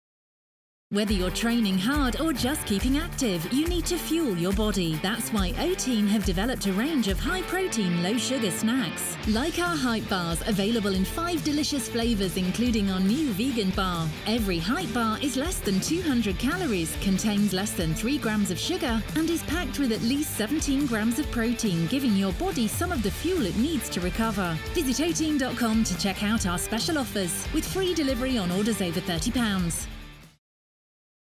Oatein BELIEVETHEHYPE on SKY TV
High Protein, Low Sugar protein bars from Oatein. Now including our brand new Vegan Chocaholic HYPE Protein Bar